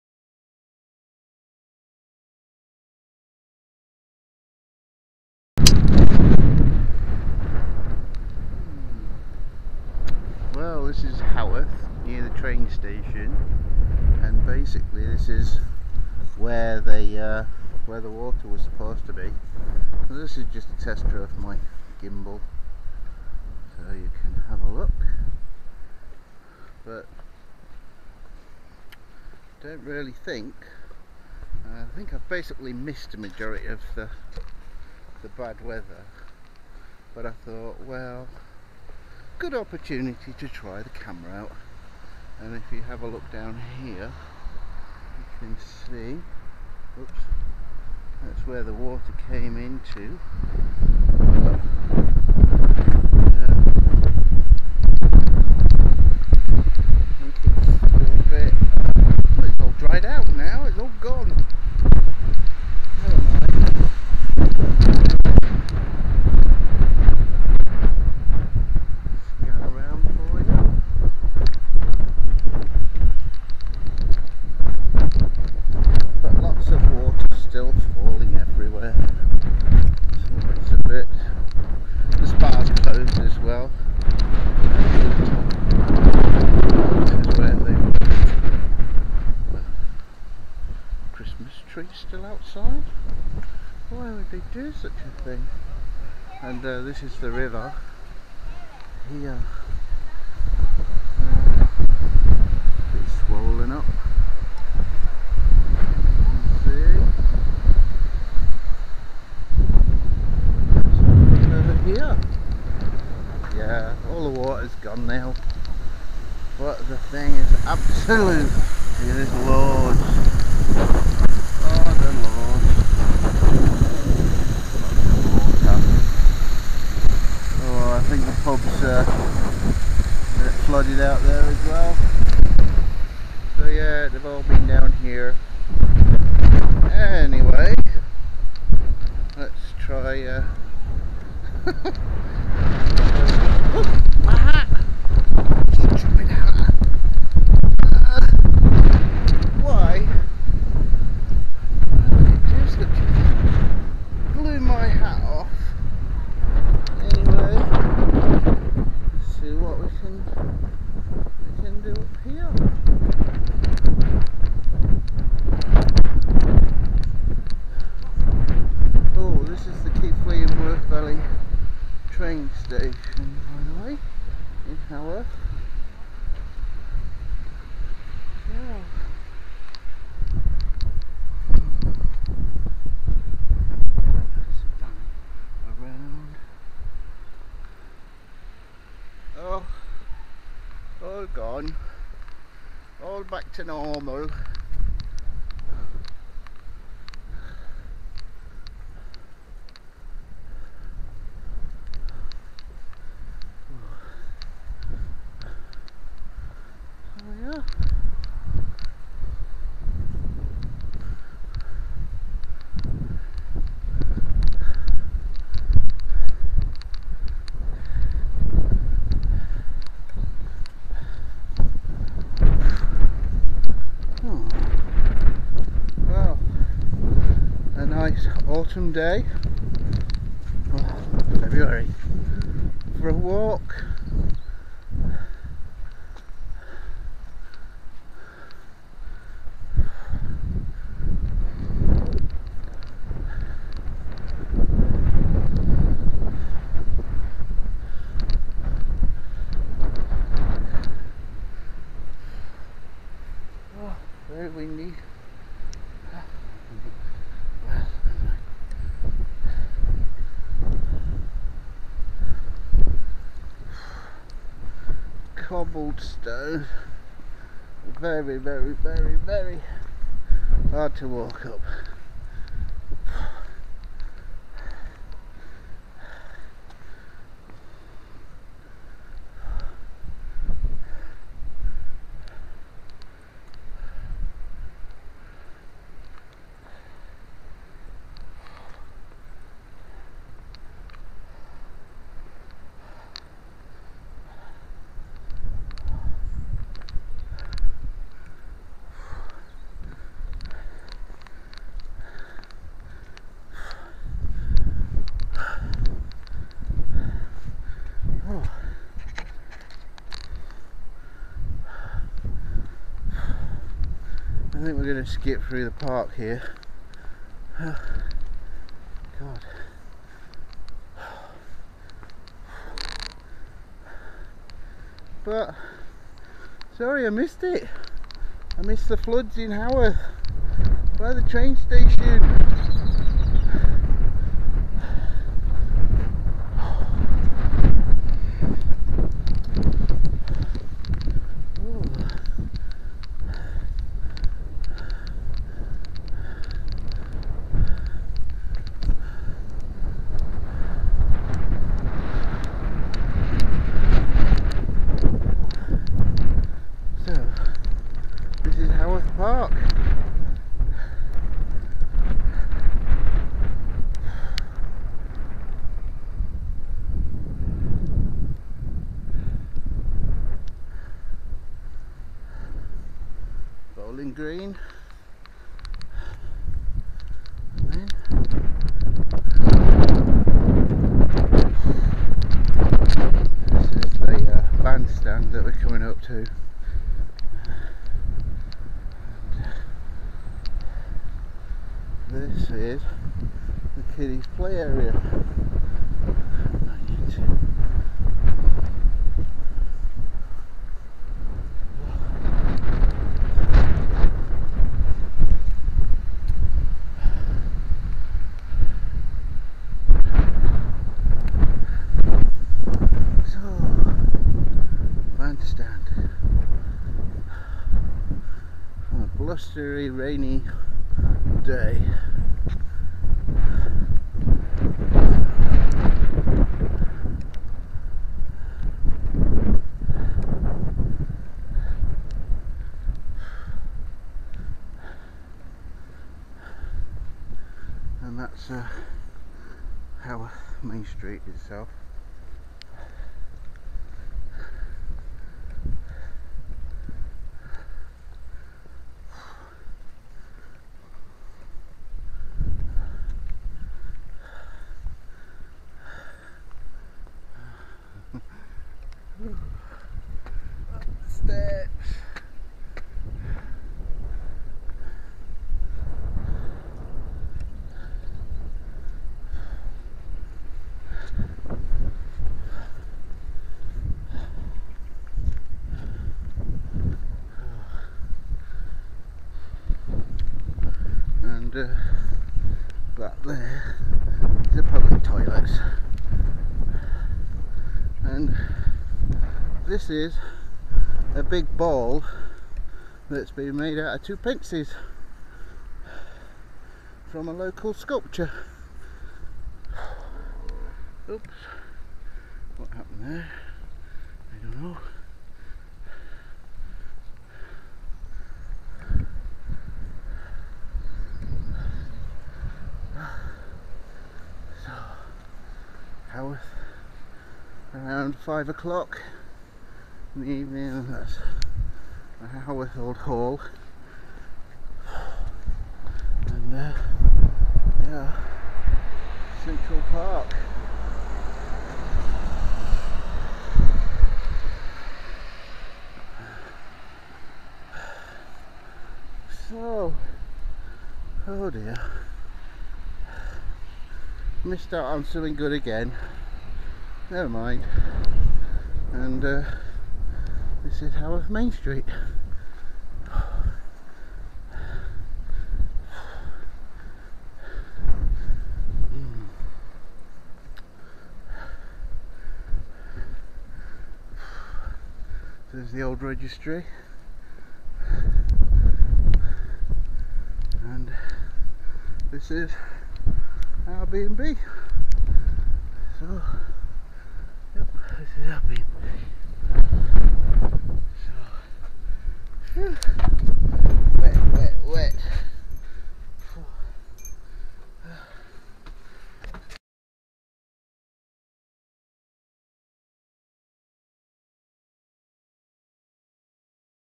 Haworth on a winters day
Also trying out my new gimbal, think it worked well and not too many shakes, also got up too late to blog the floods sorry.